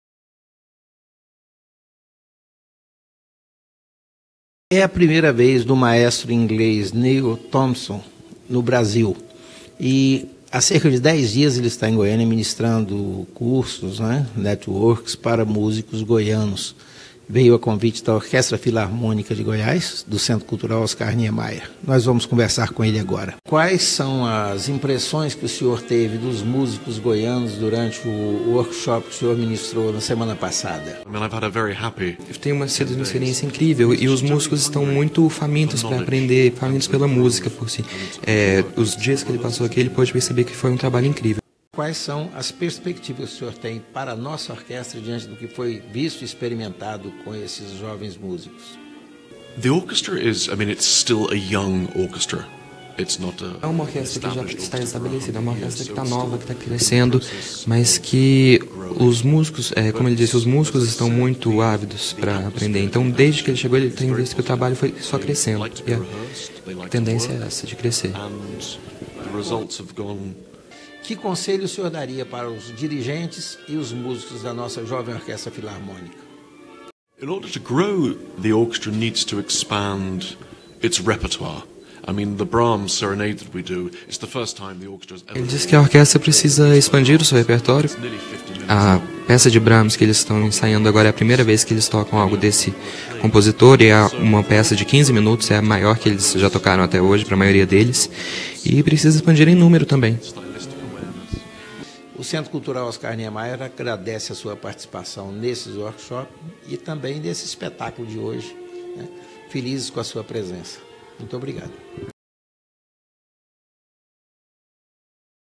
OFG - Regente convidado Neil Tomson
Exclusiva ao Centro Cultural Oscar Niemeyer do regente britânico convidado pela Orquestra Filarmônica de Goiás no dia 23 de Outubro de 2012 para a série "Grandes Solistas", no teatro SESI de Goiânia, Goiás.